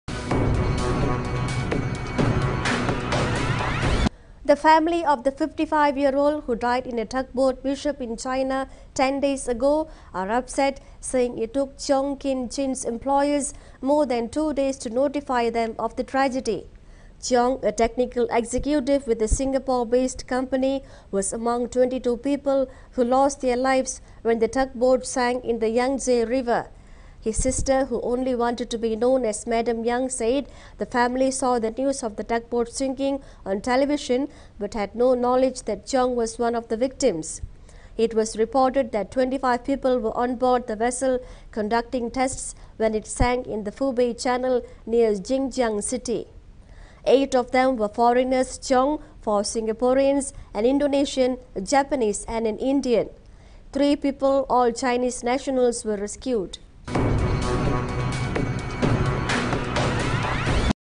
News of Malaysia's Death In China Tugboat Tragedy Reached Family Late
The family of the 55 year old who died in a tugboat mishap in China 10 days ago are upsets, saying it tokk Cheong Kin Chin's employers more than two days to notify them of the tragedy.